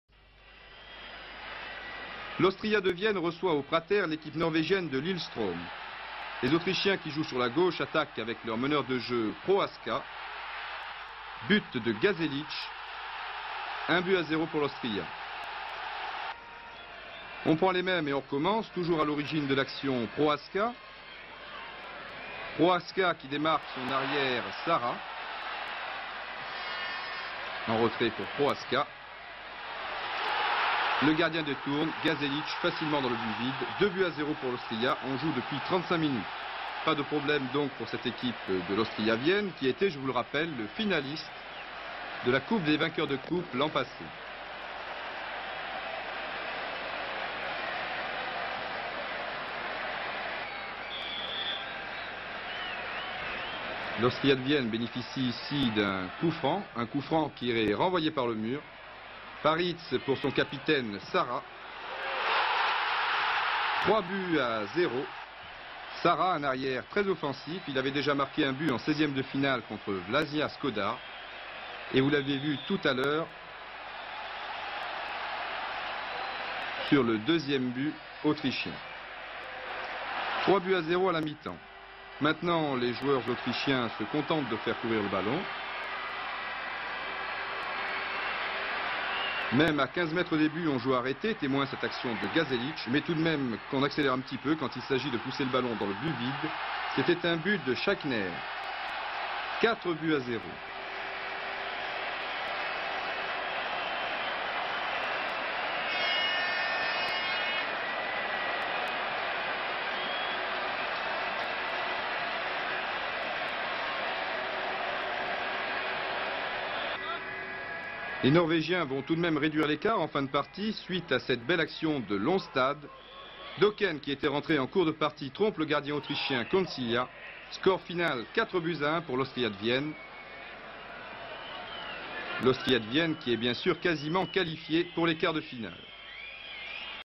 Austria Vienna-Lillestroem 4-1 Coppa Campioni 78-79 2' Turno Andata
25' e 34' Gasselich
36' Sara
62' Dokken (LI)
65' Schachne